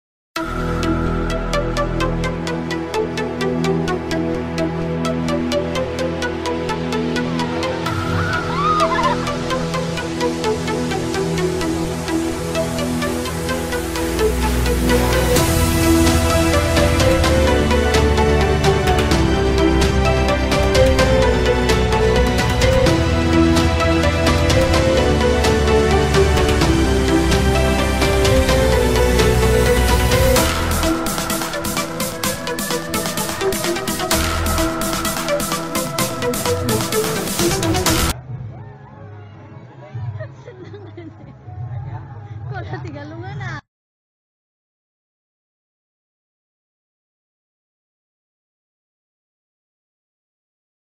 Baby orangutan locked in cage in appalling conditions (Bayi orangutanl)
Baby orangutan confined in small cage in terrible conditions serving as zoo attraction. Orangutans are not pets.